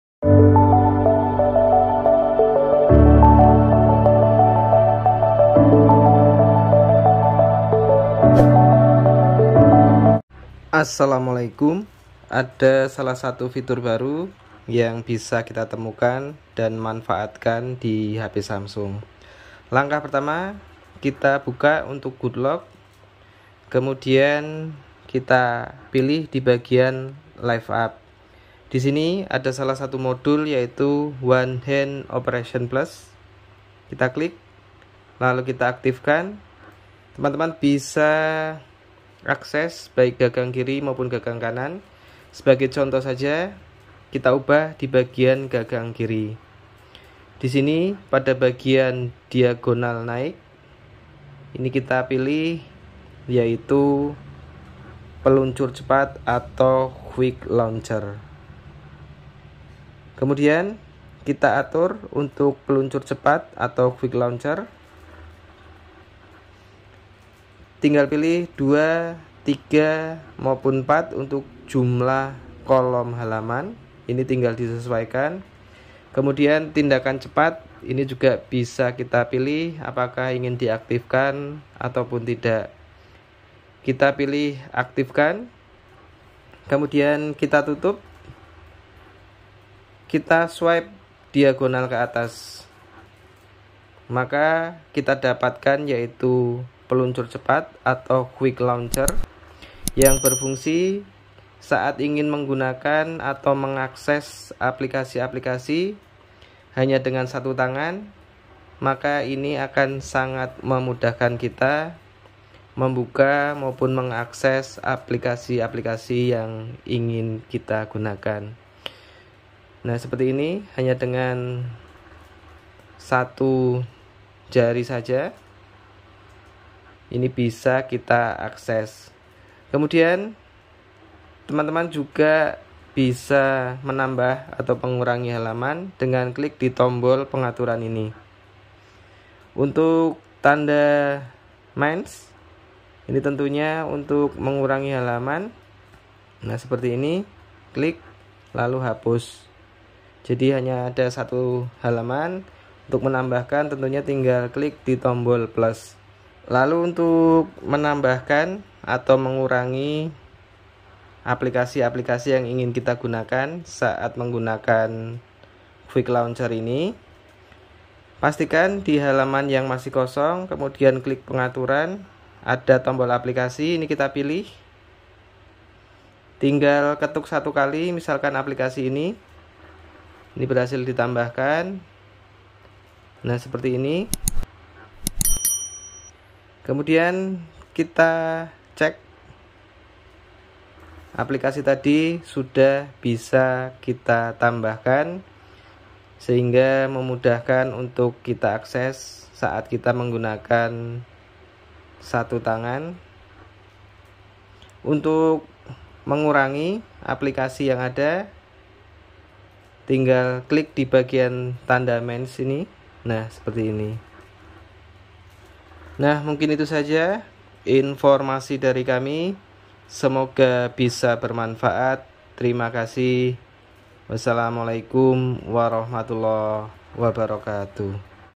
Fitur Baru Bermanfaat Di HP Samsung - Quick Launcher
Fitur Baru Di HP Samsung Untuk One Hand Operation +.